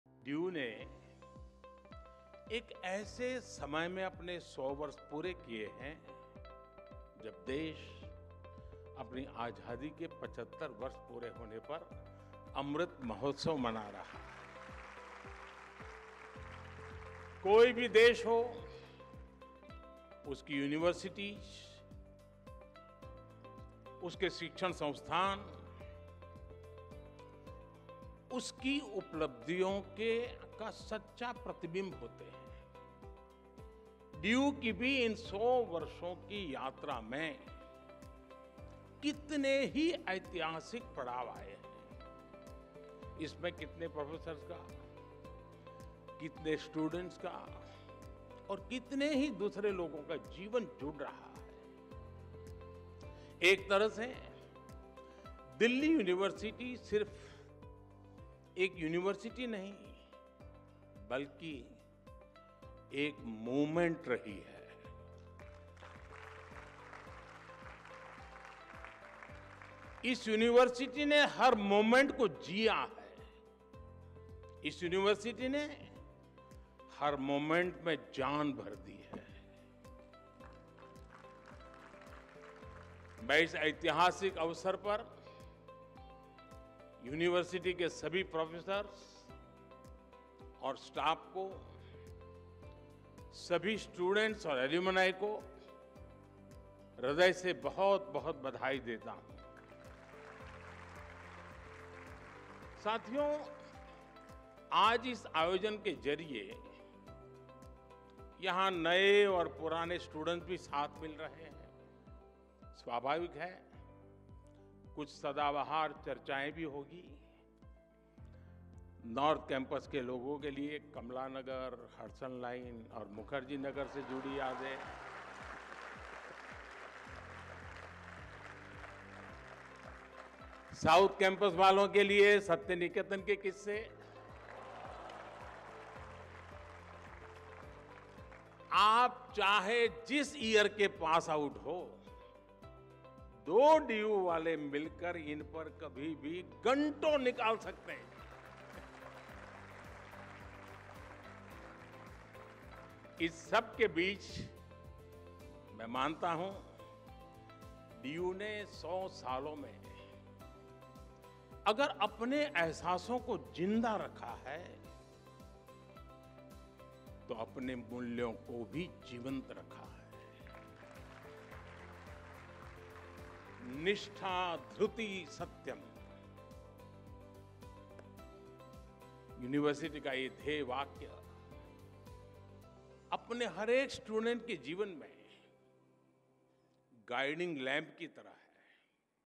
DU Not Just A University, But A Movement
Delhi University has completed its 100 years at a time when the country is celebrating Amrit Mahotsav to mark 75 years of its independence. Be it any country, its universities, educational institutions are the true reflection of its achievements. In the journey of 100 years of DU too, there may be many historical milestones: Prime Minister @NarendraModi